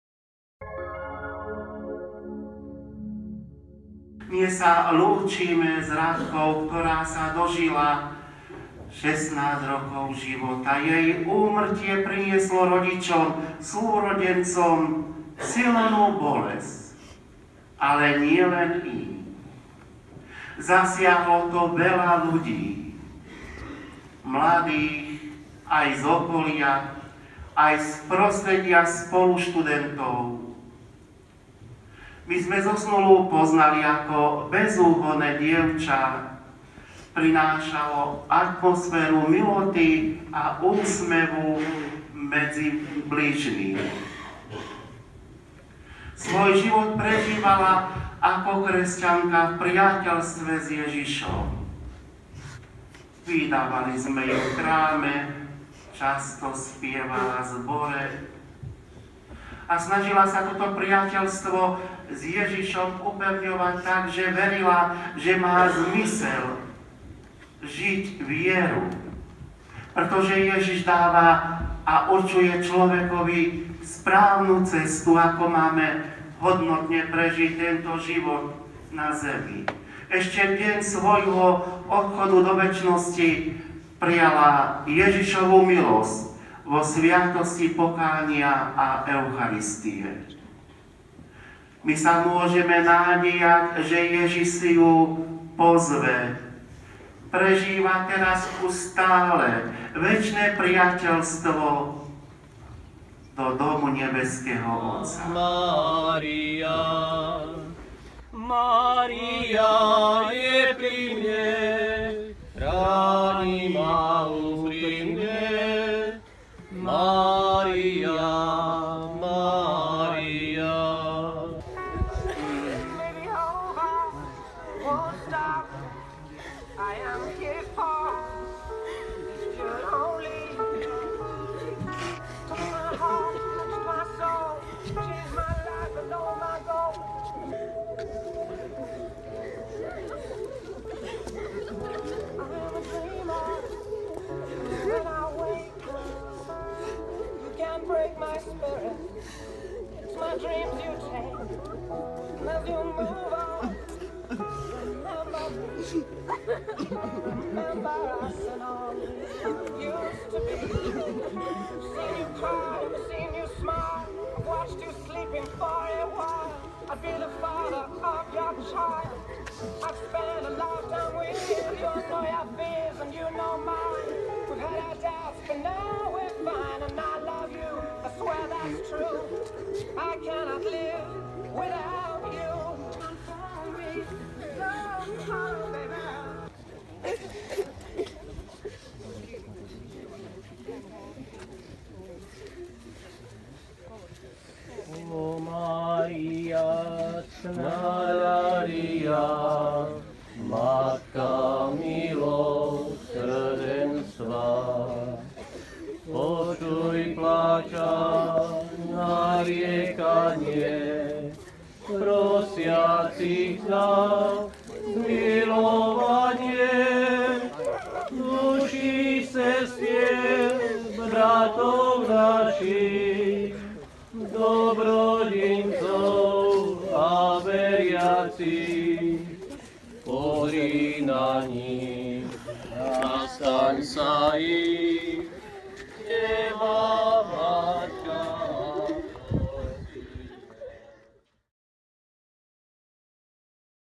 Posledná rozlúčka s Radkou z Oravy
S Radkou, ktorá tragický zahynula sa rozlúčila rodina, najbližší, spolužiaci a kamaráti